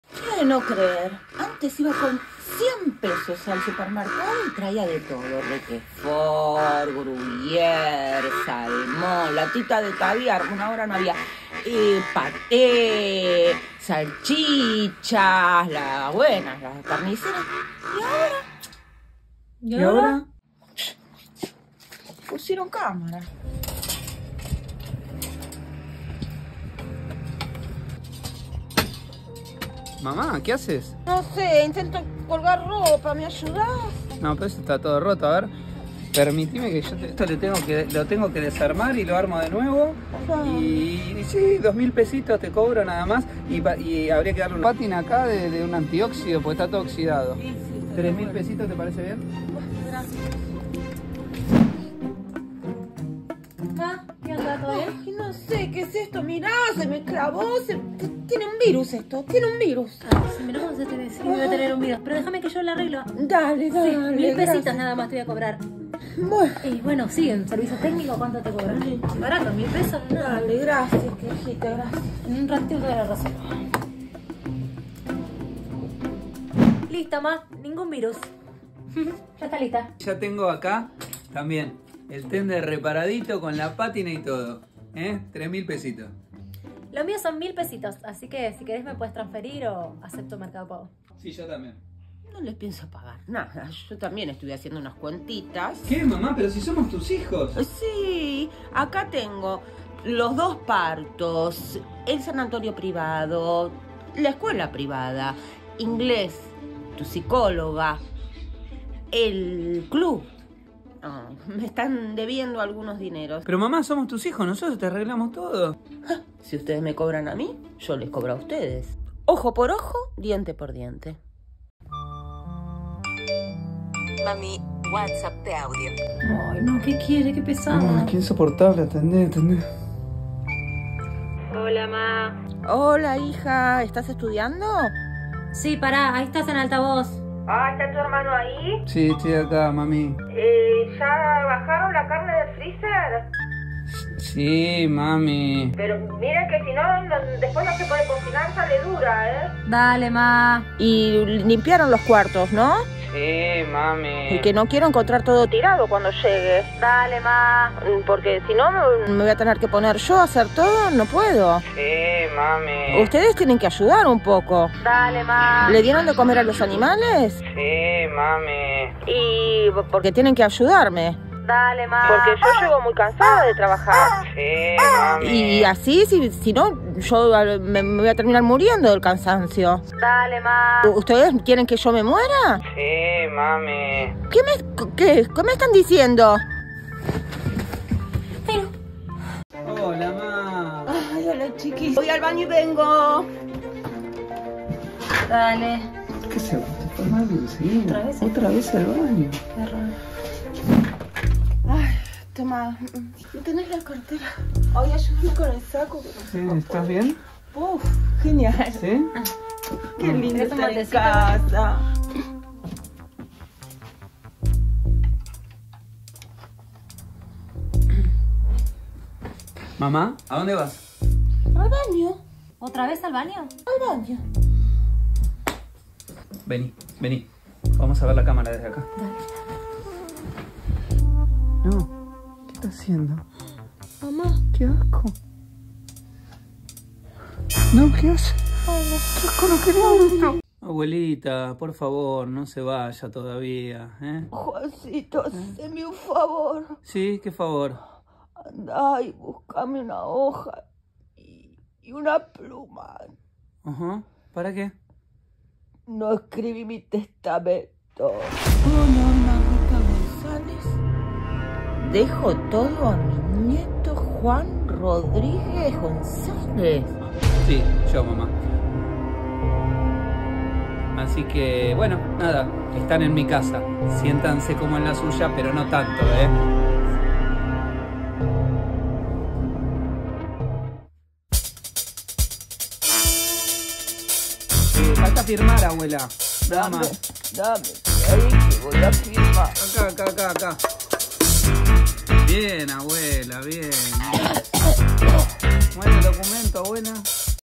Una madre y sus dos hijos; situaciones cotidianas de una familia.
Bienvenidos a nuestra Realidad Amorosa donde proliferan todas las historias.
Una hermosa joven, Yamila, trabaja en la casa de un joven Juan y su Madre. Humor, suspenso drama y un refinado erotismo. Yamila la mucama tiene de todo!